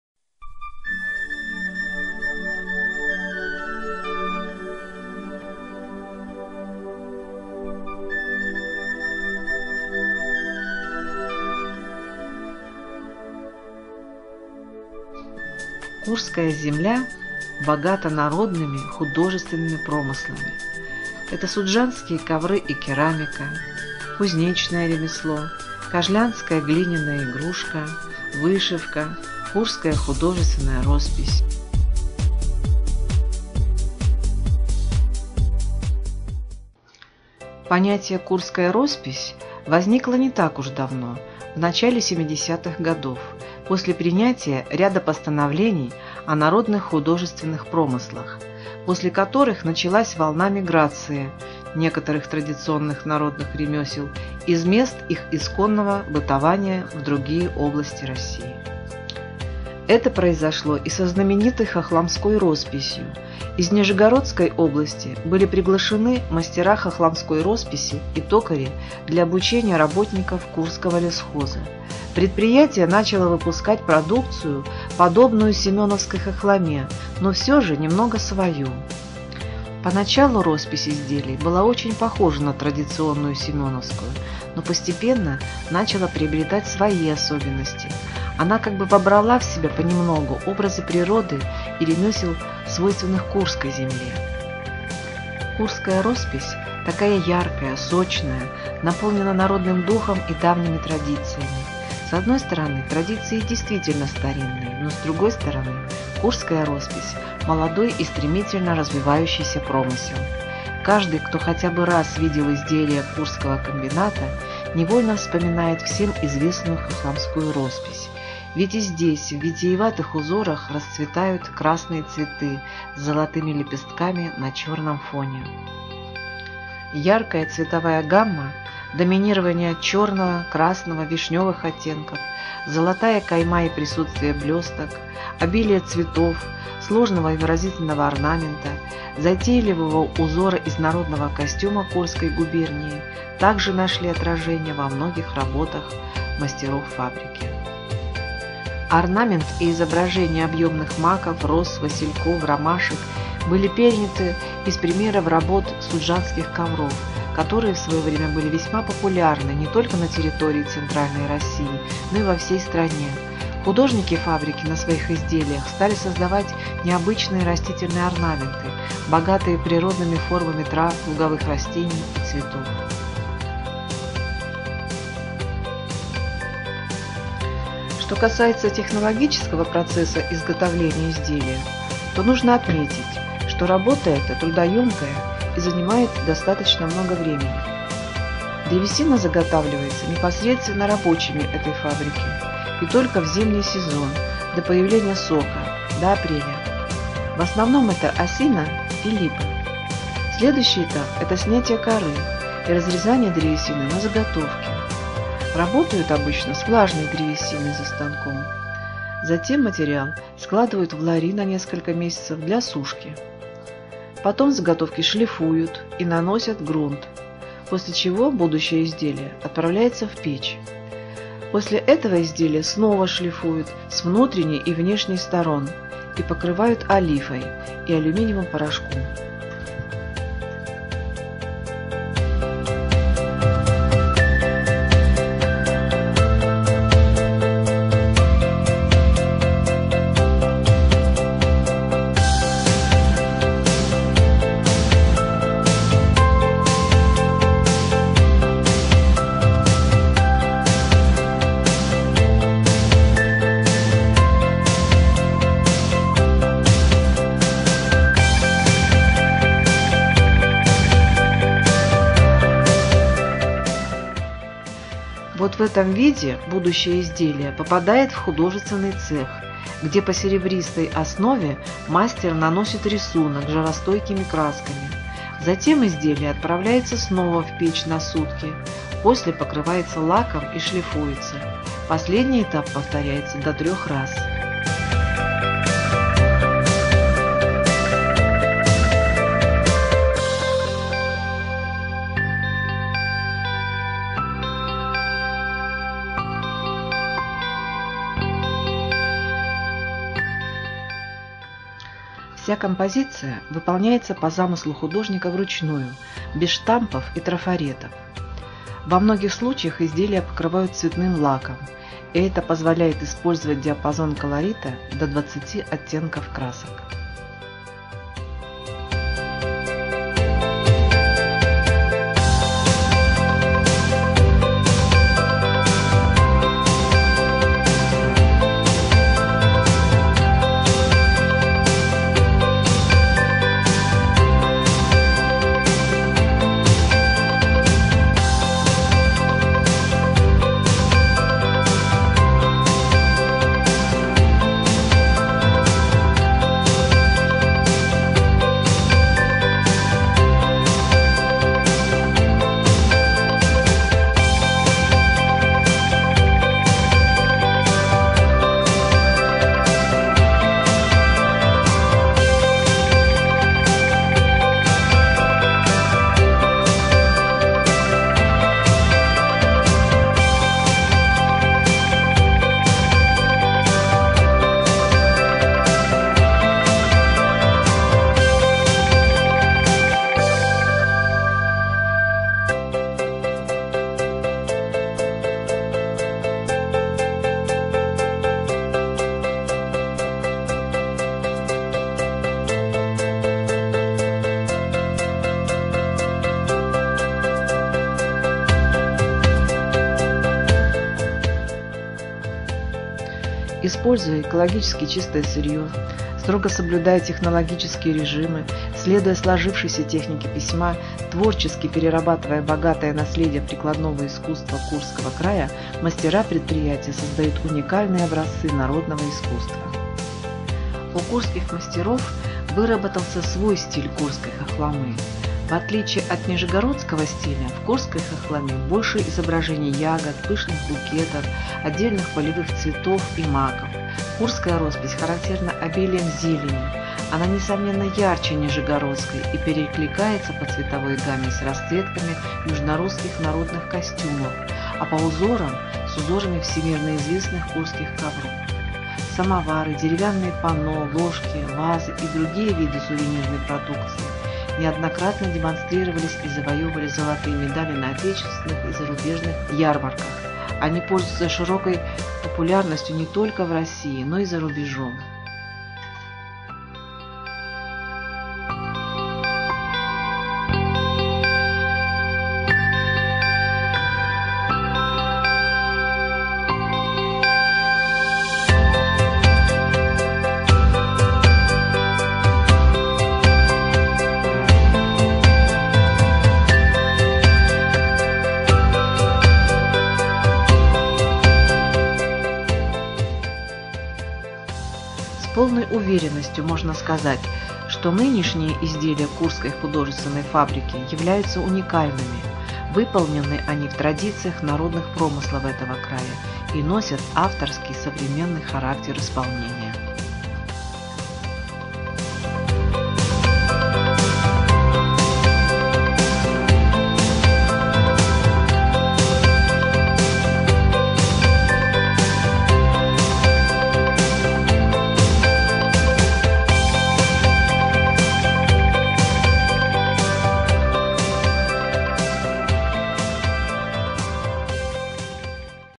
Kursk "Khokhloma", Kursk painting. Курская "хохлома", курская роспись.
Видеофильм был сделан под впечатлением, после посещения Курского художественного комбината по производству художественной росписи по дереву, который был создан в 1972 году, и хотя, роспись тоже называется хохломой, (т.к. первые преподаватели были из Нижегородской области), но по сравнению с нижегородским популярным промыслом, у курской росписи выработаны свои особенности – обилие ягод, пышных букетов, полевых цветов и маков. Курская роспись ярче по цветовой гамме, а узоры как на знаменитых курских коврах. Курскому художественному комбинату присвоен статус «Предприятие народных художественных промыслов».
The video was impressed after visiting the Kursk Artistic Plant for the production of art paintings on wood, which was created in 1972, and although the painting is also called khokhloma (since the first teachers were from the Nizhny Novgorod region), but in comparison with Nizhny Novgorod popular craft, the Kursk painting has its own characteristics - an abundance of berries, lush bouquets, wildflowers and poppies. Kursk painting is brighter in color, and patterns as on the famous Kursk carpets. The Kursk art factory was given the status "Enterprise of folk art crafts".